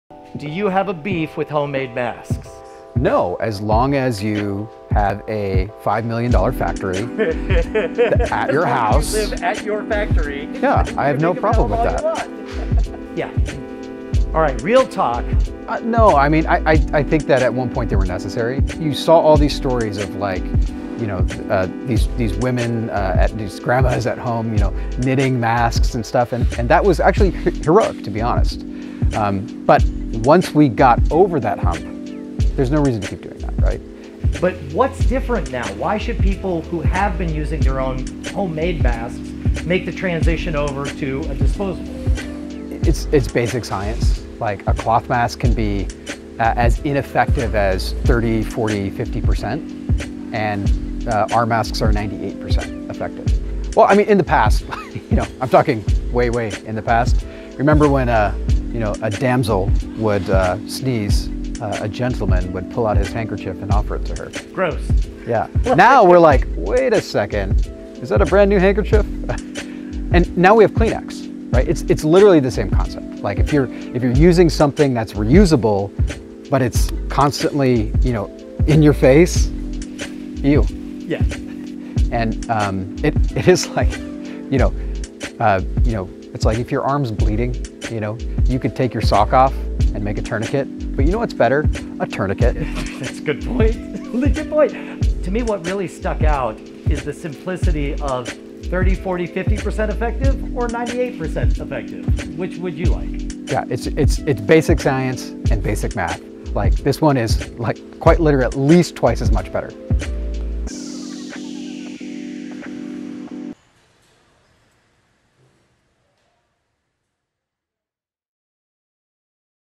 Surgical vs Homemade Masks
When the pandemic hit our country didn't have enough PPE on hand for doctors...let alone for people at home. Americans came together to start making their own cloth masks at home from materials on hand. That was a phenomenal stop gap measure, but it was never supposed to last forever. Let's explore why in 2021 everyone can and should be wearing real masks.